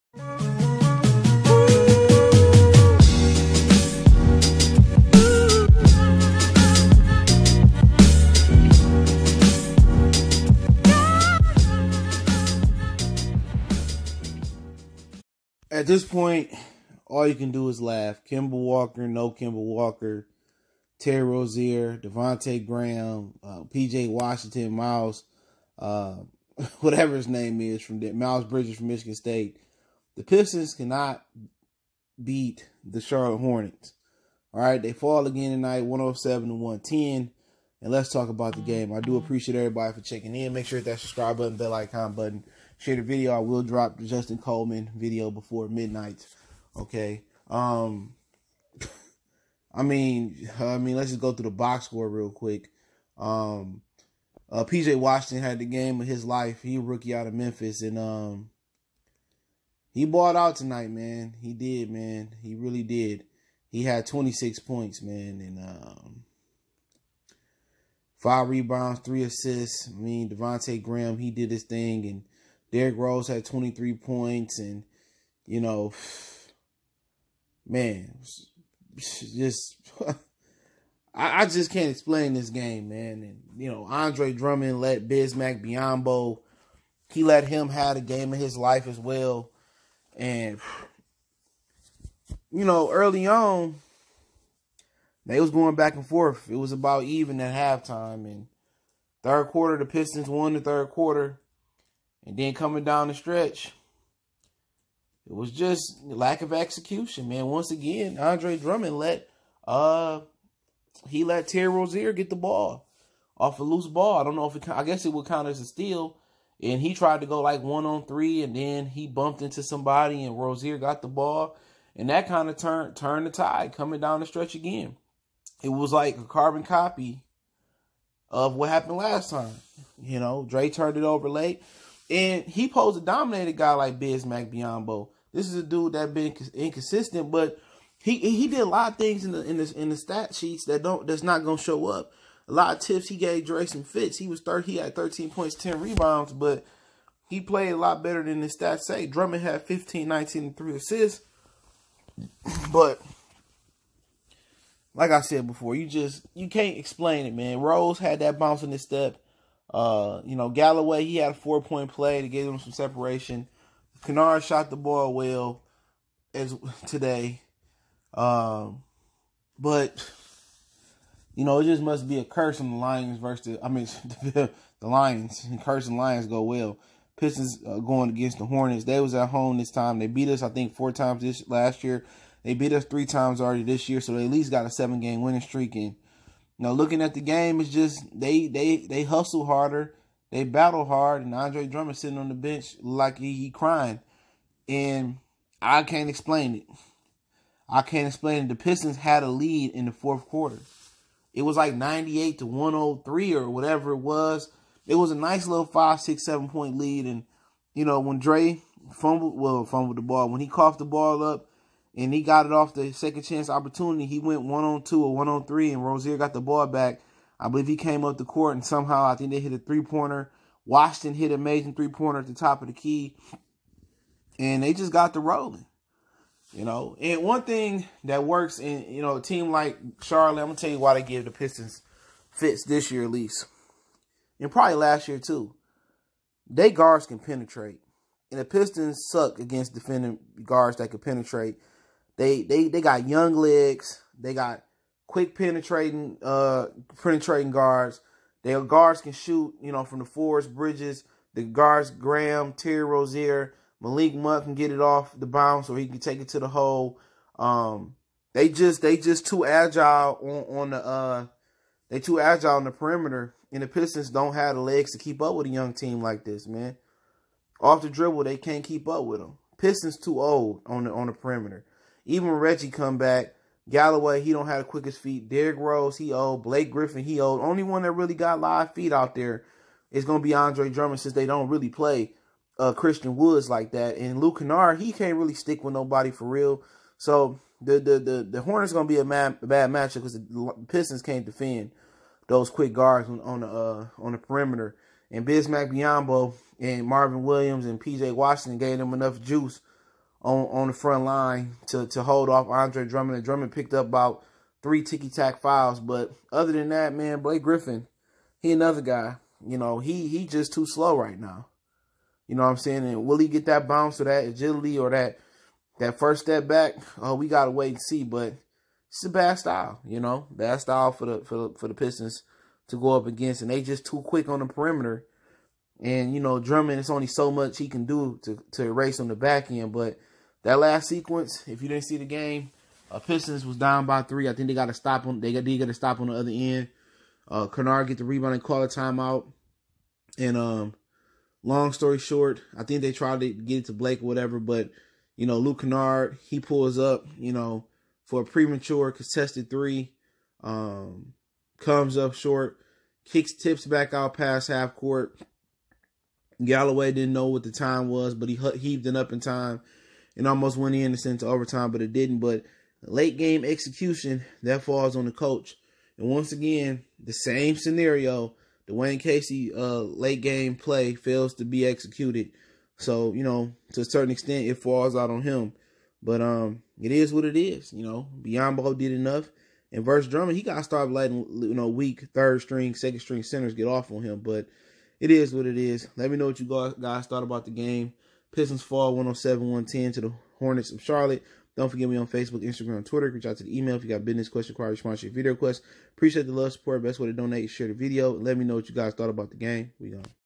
Detroit Pistons Lose Again to Charlotte Hornets 107-110 | Full Game Reaction Video!!!
CjGood313